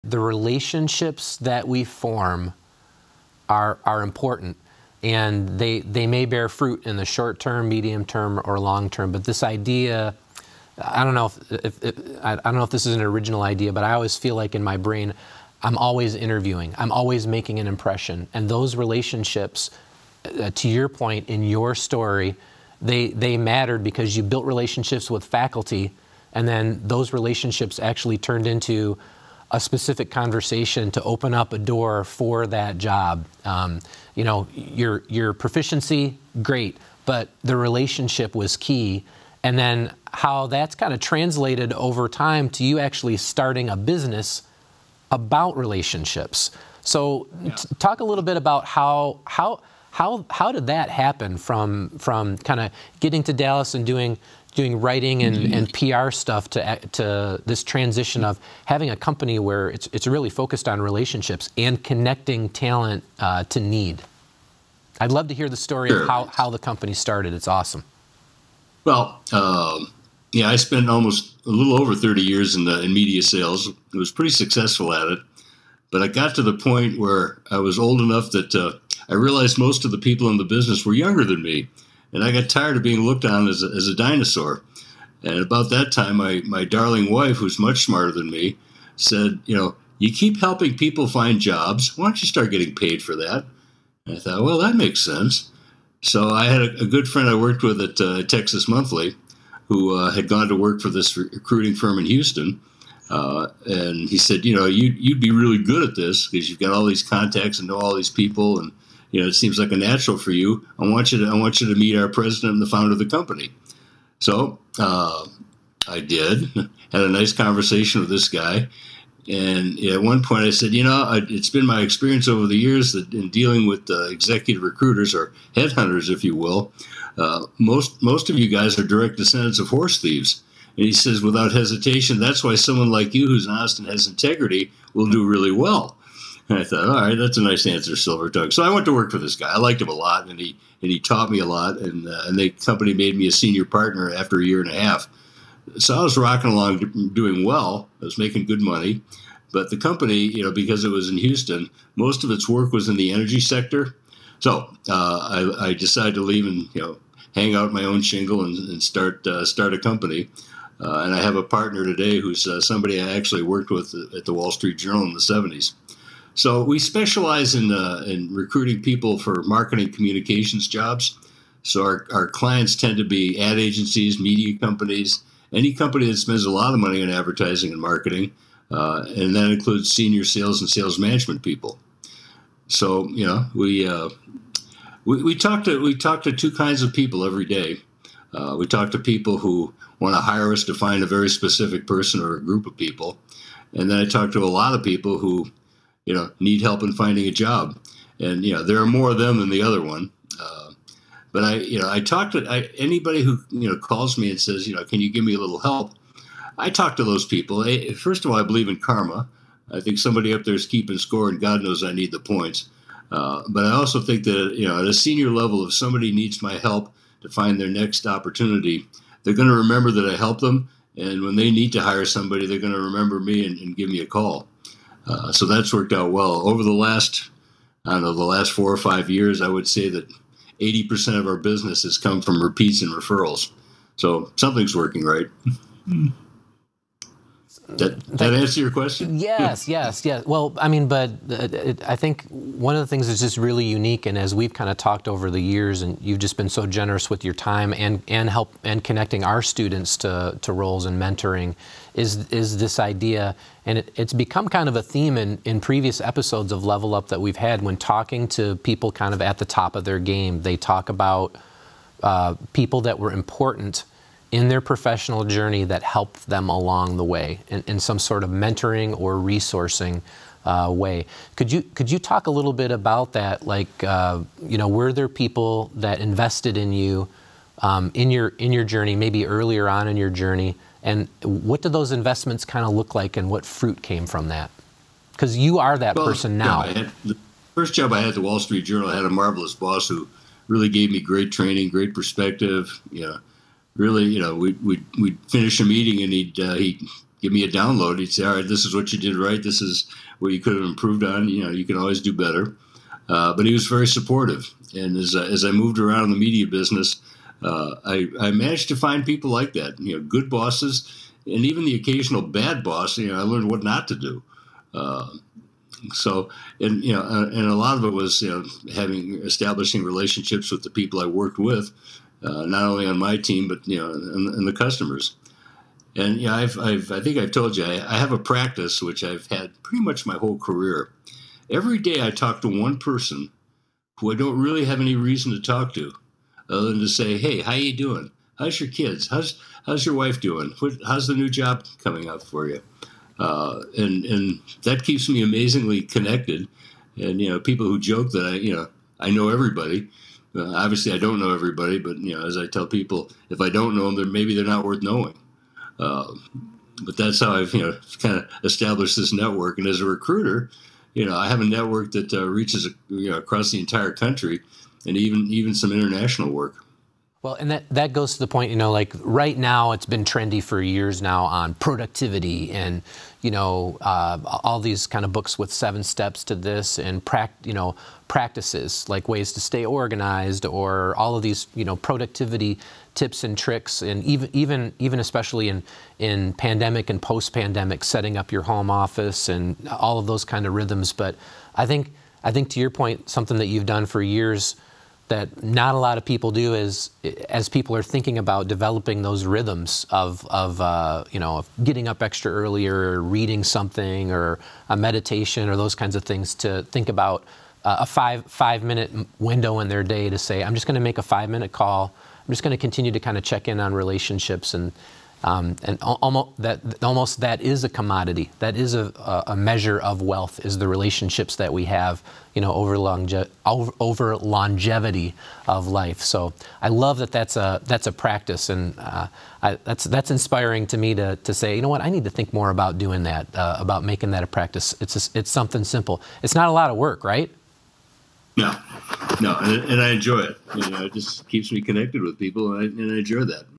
Segment Two: Relationships are Key
Presenter: Roger Tremblay, Executive Recruiter & Founding Partner, PointClear Search
Host: Jason Archer, Director, Strategic Communications M.A. for Michigan State University

Session Overview: "Resumes and Relationships: A recruiters advice on successful job searching"